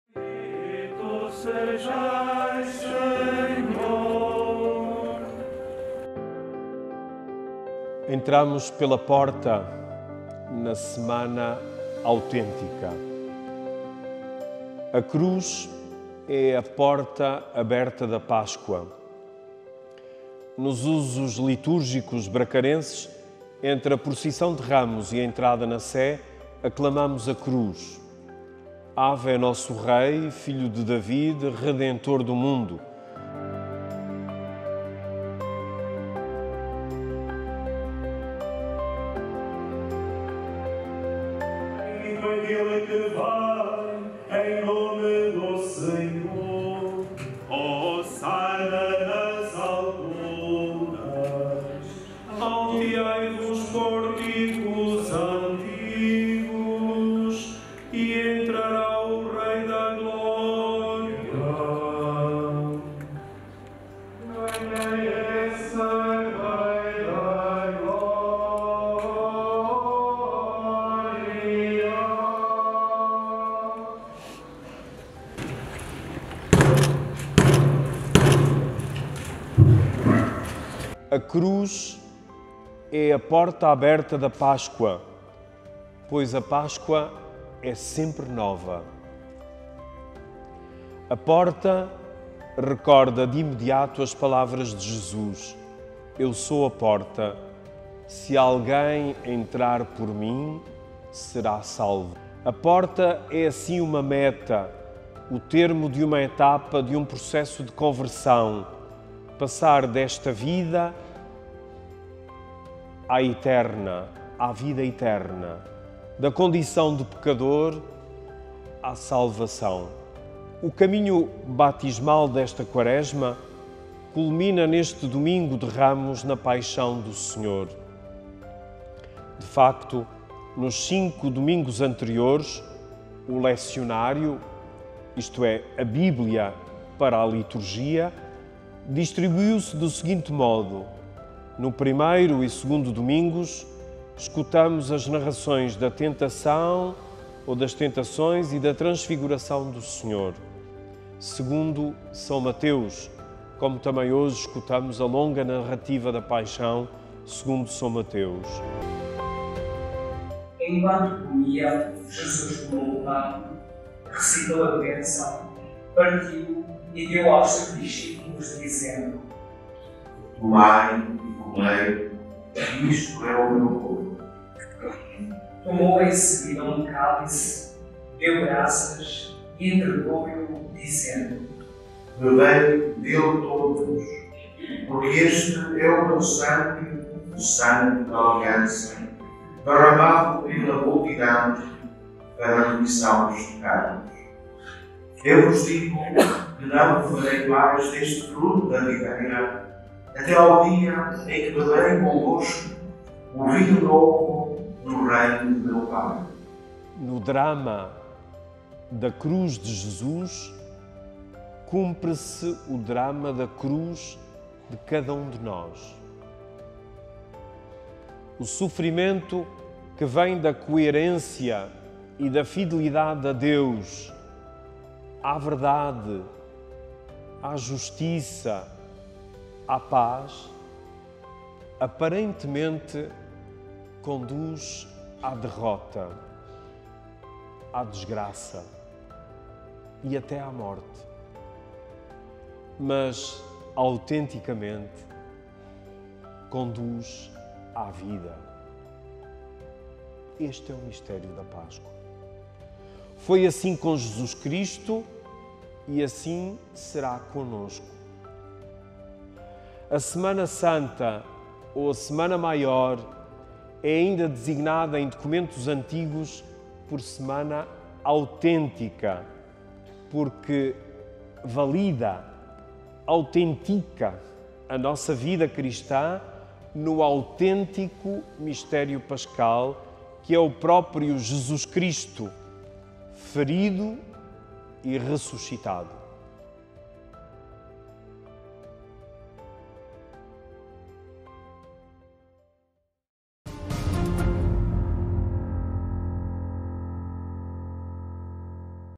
Domingo de Ramos - Semana Santa de Braga 2023
A Benção e Procissão dos Ramos, com a abertura da porta da Catedral, segundo o Rito Bracarense, realizou-se na manhã do Domingo de Ramos. Recorda alguns momentos.